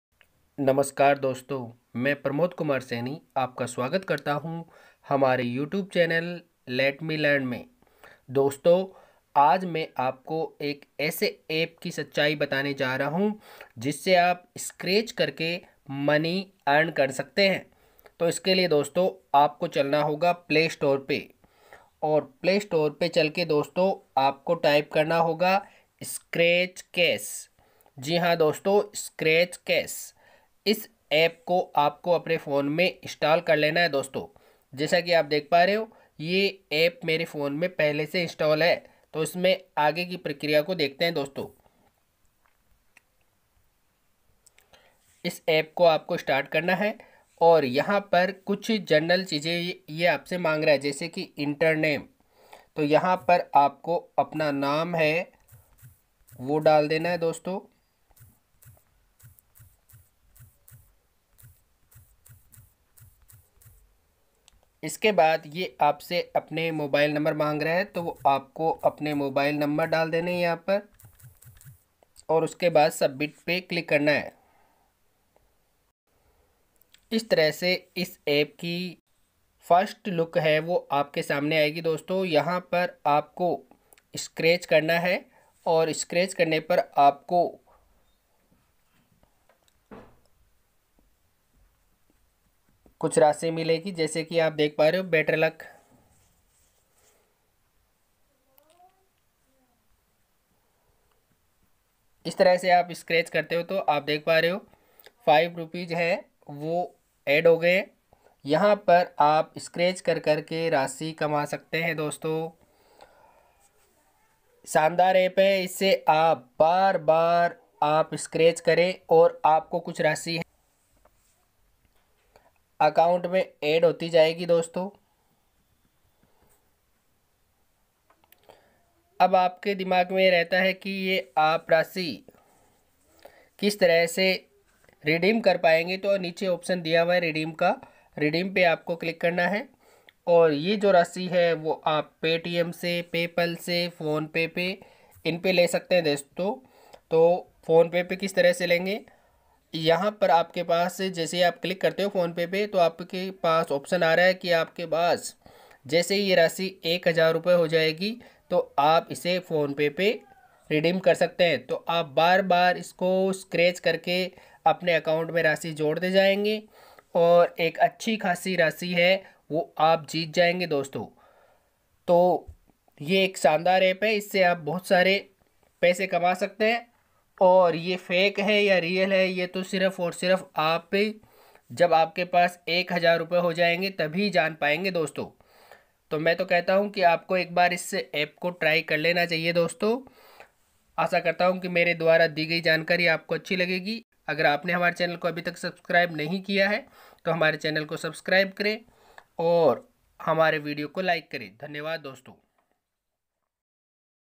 Scratch and Earn
नमस्कार दोस्तों
इस वीडियो में आपको एक बहुत ही आसान और सरल तरीका बताया जाएगा कि कैसे आप स्क्रैच करके भी धन कमा सकते है।
हमारा ये वीडियो Scratch Cash एप्प की पोल खोलेगा।